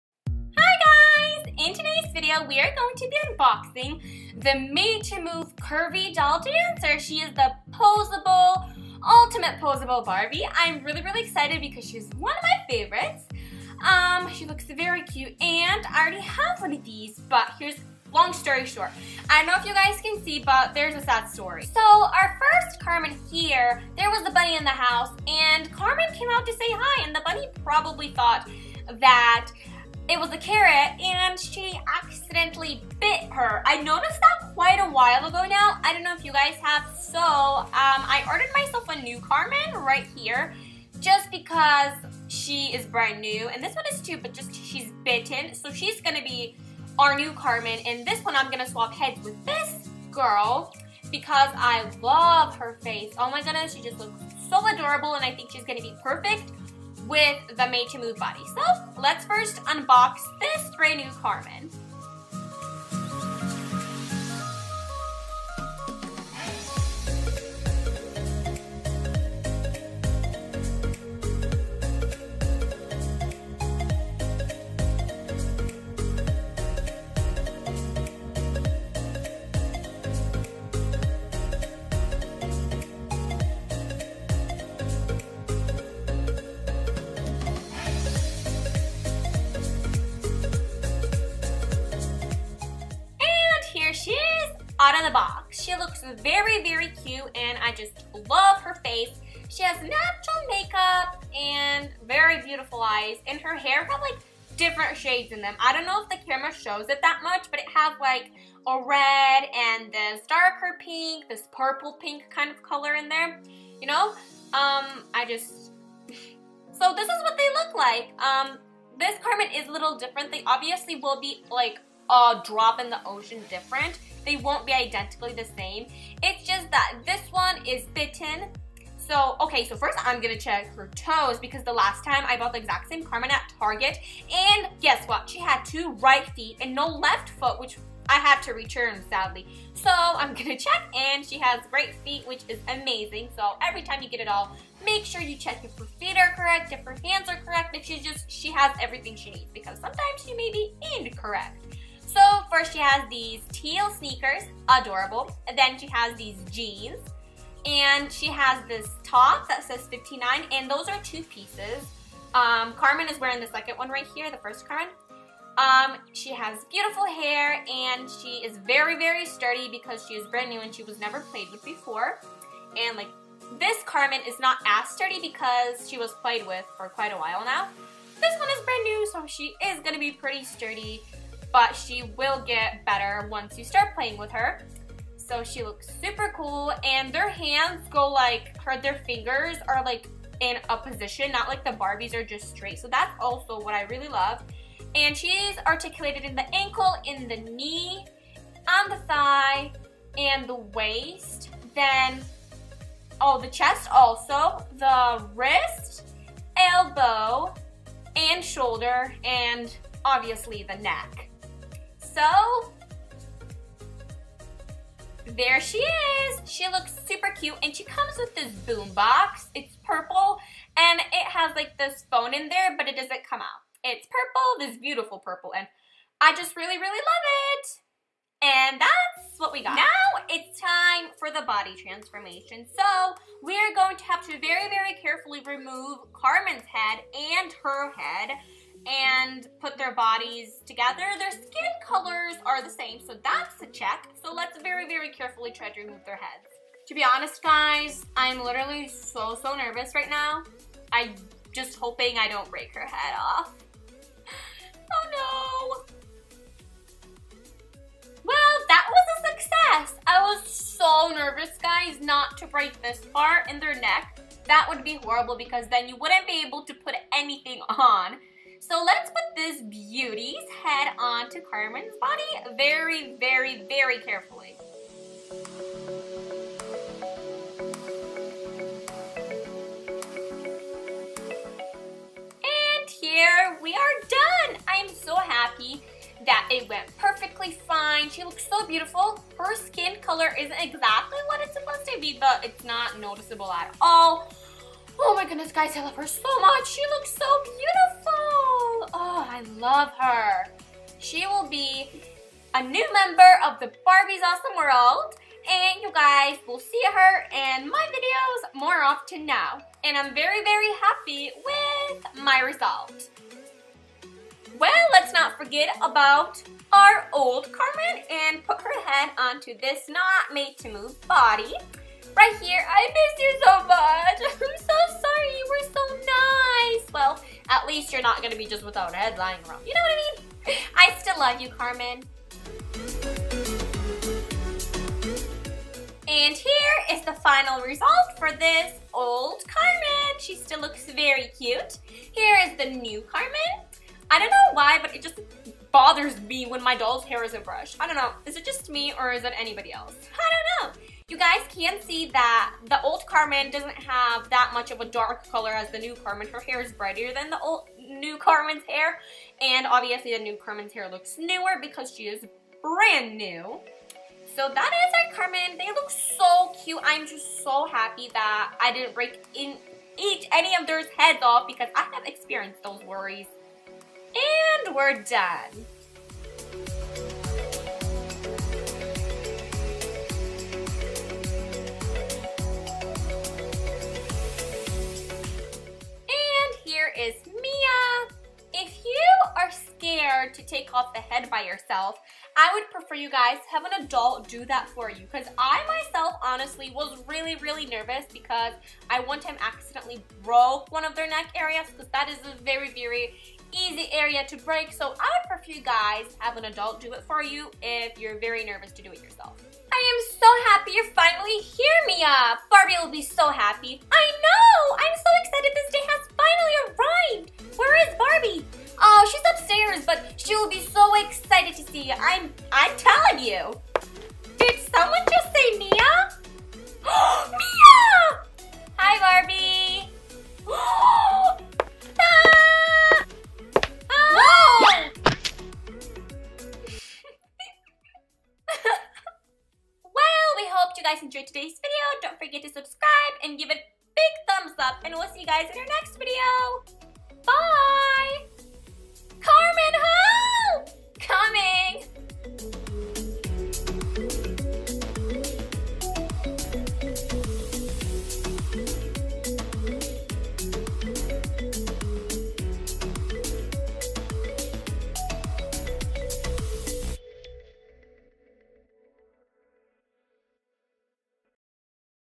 Unboxing the made to move Barbie Doll Dancer
Hey guys in today's video we are unboxing the made to move Barbie doll dancer hope you guys enjoyed today's video!!!😆